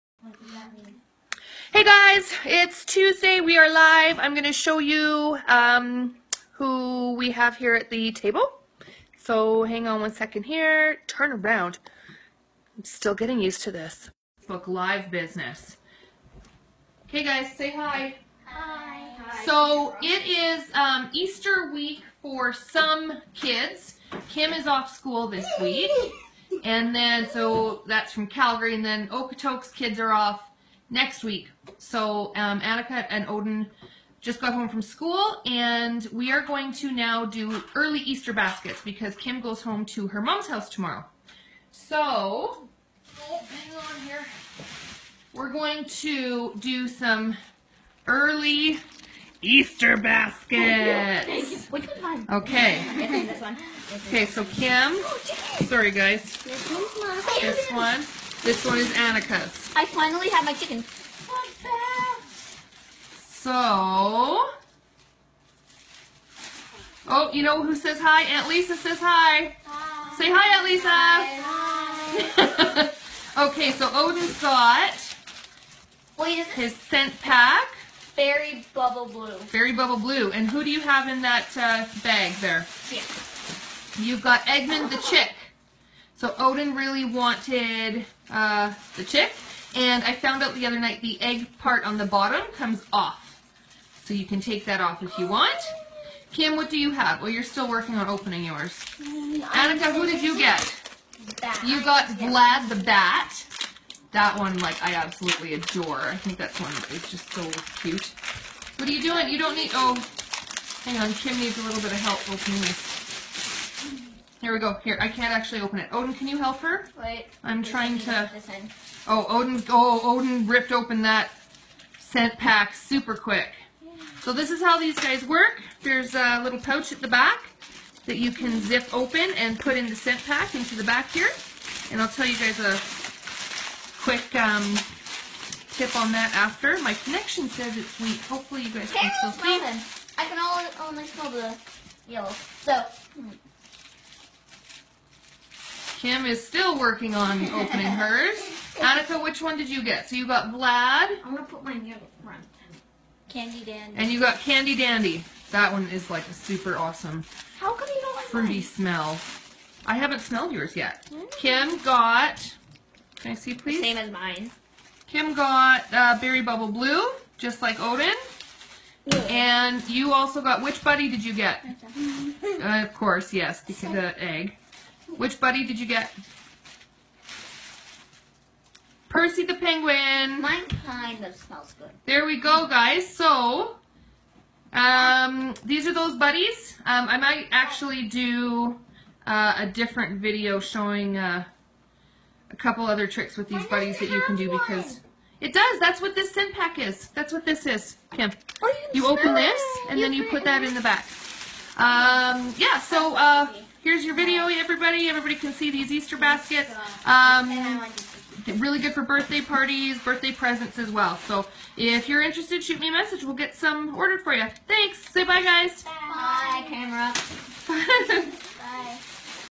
Kids opening Scentsy buddies!
My kids opening their Scentsy Easter baskets!

Eggmund the Chick
Vlad the Bat
Percy the Penguin

Scent packs are Candy Dandy and Berry Bubble Blue